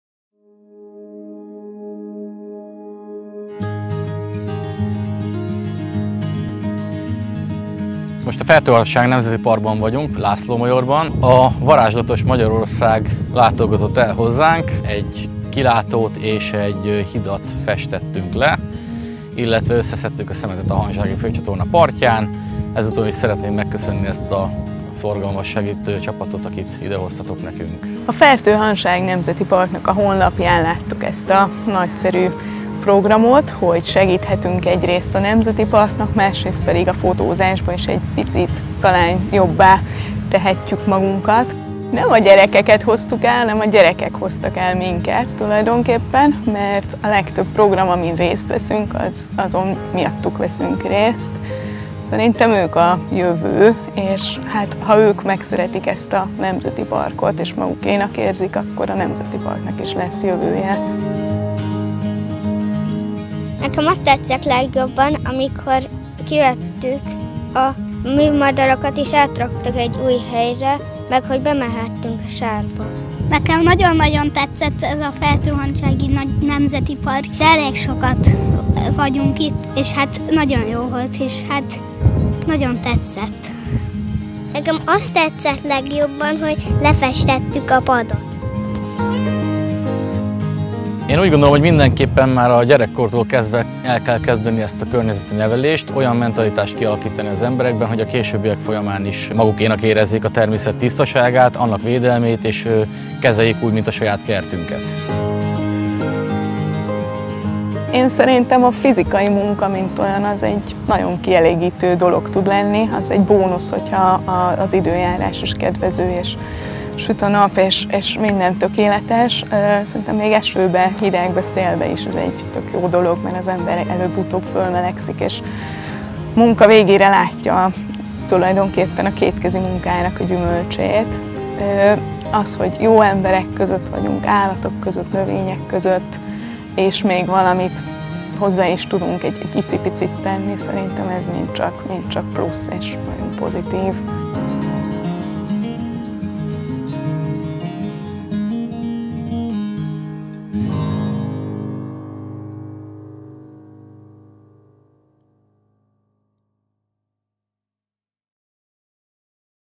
Fertő-Hanság Nemzeti Park - Ma egy fészek, holnap az óceán! - 2020
Az ország egyik legszebb vidékén, a Fertő-Hanság Nemzeti Parkban kalandoztunk természetbarátainkkal. A szeles, de csodaszép napfényes őszi napon a Fertő szikes tavainak tanösvénye mentén segítettünk felújítani egy hidat és menedékházat, majd távolból s közelből megtekintettük a helyi élővilágot!

Köszönjük az önkéntesek munkáját, és a Fertő-Hanság Nemzeti Parknak hogy ott tölthettünk egy szép napot!

Köszönet a Lenergy Magyarországnak a Cluster Medianak és az Öko-Terméknek a támogatásért, illetve a Karnevál Filmnek a képekért és filmért